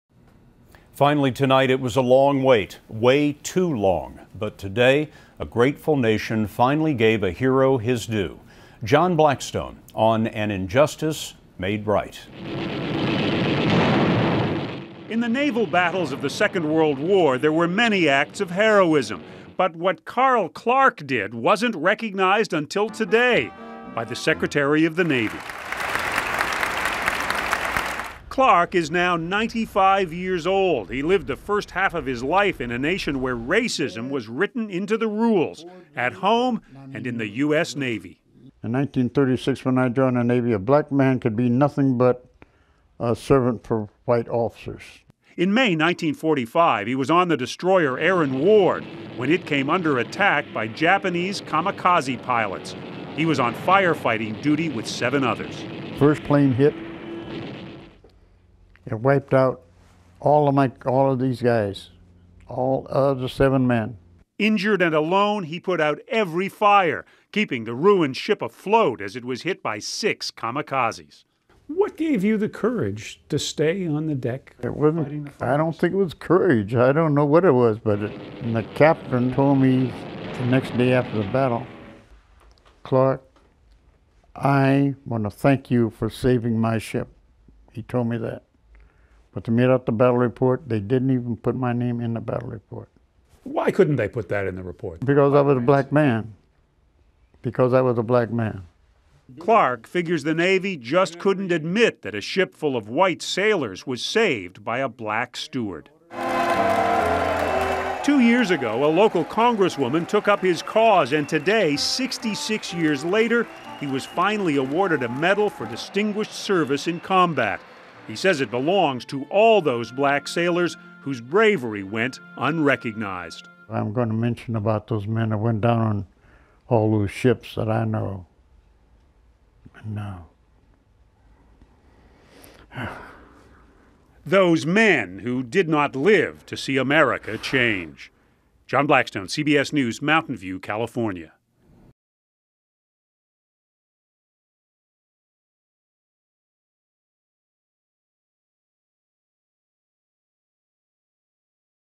WW II vet honored after 66 years
In 1945, Carl Clark saved the Navy destroyer ship Aaron Ward from Japanese Kamikaze pilots. But he was not recognized for his heroism because of his race. Now, 66 years later, Clark was awarded his Medal for Distinguished Service in Combat. John Blackstone reports.